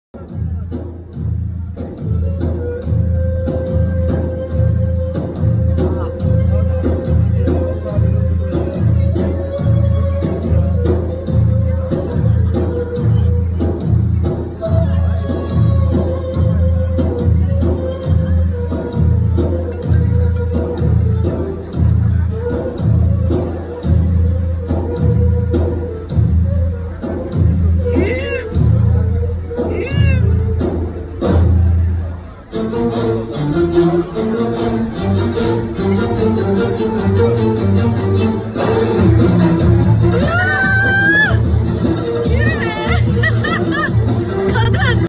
Ebru Polat'ın dans videosu sosyal medyayı salladı!
Her paylaşımı olay olan ünlü şarkıcı Ebru Polat'ın dans videosu sosyal medyayı yerinden oynattı. Paylaşıma kısa süre içinde binlerce beğeni ve yorum yağdı.